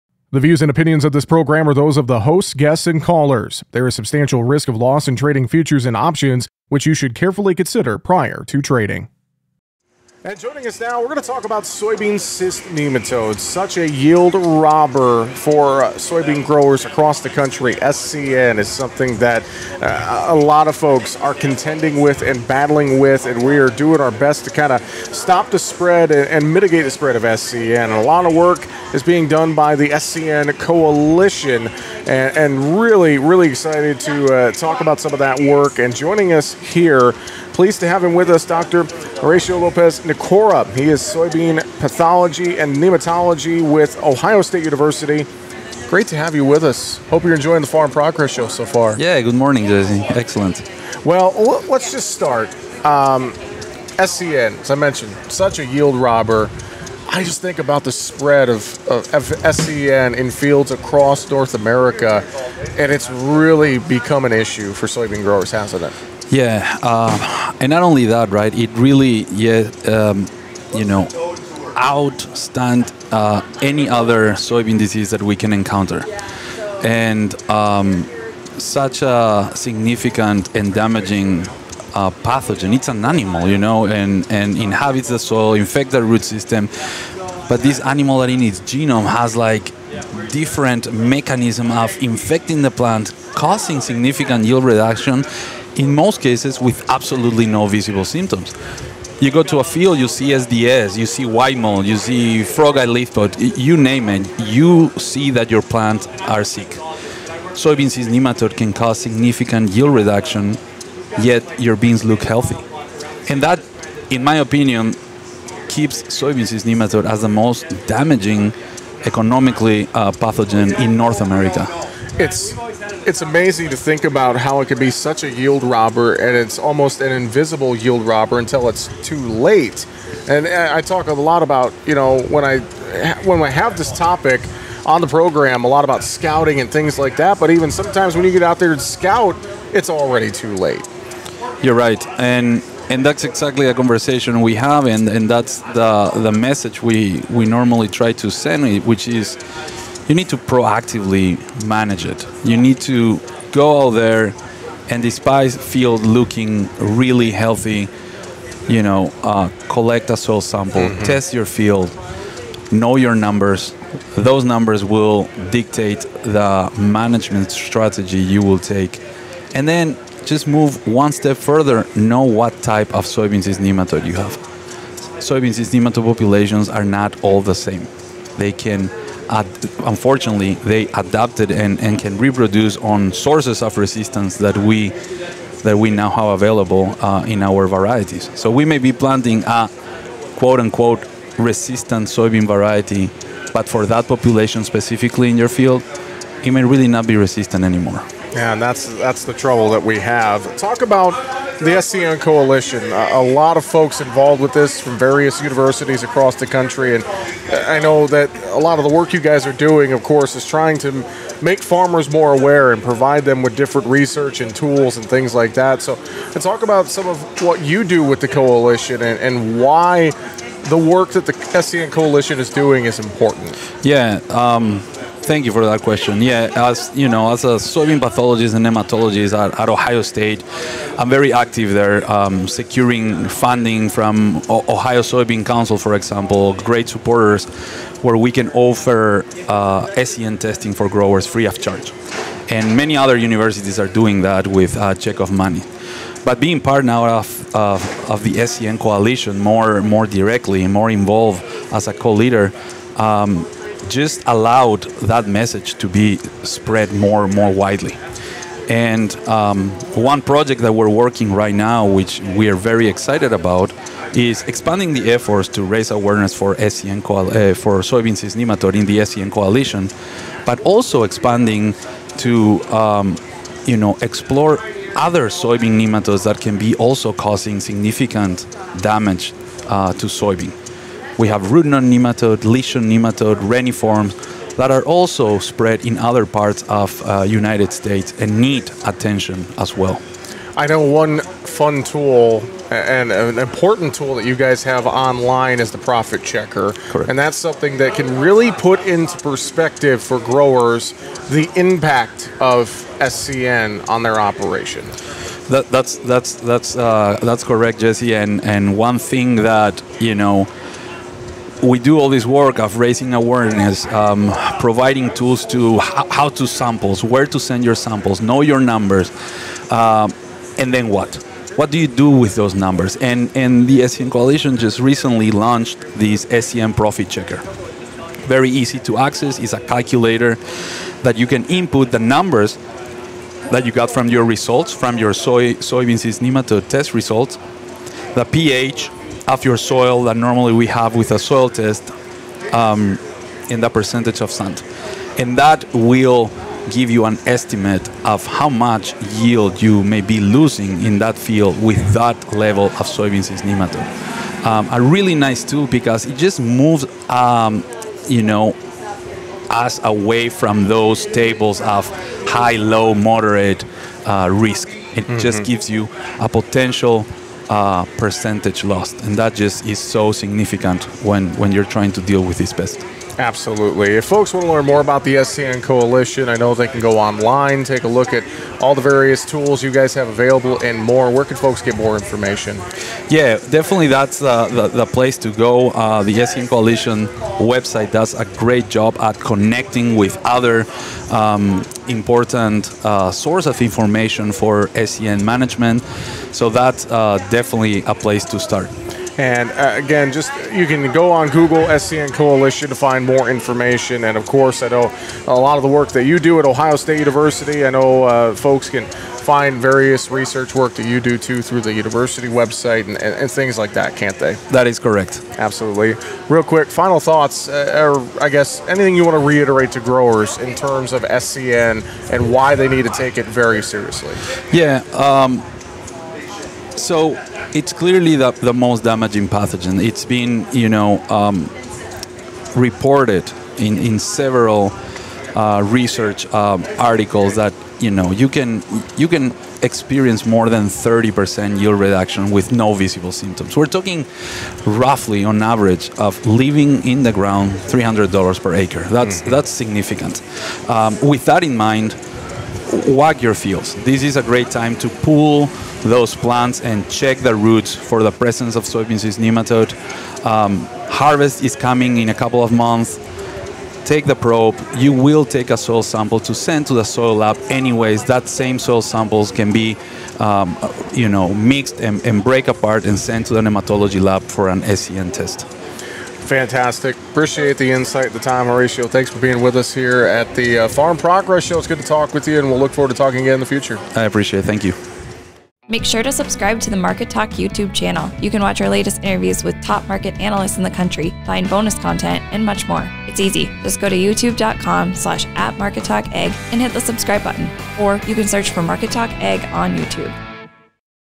The Fight Against Soybean Cyst Nematode
We discuss the work of the SCN Coalition and talk with Dr. Horacio Lopez-Nicora, Assistant Professor of Soybean Pathology and Nematology at Ohio State University about the spread of soybean cyst nematode across the U.S. in recent years.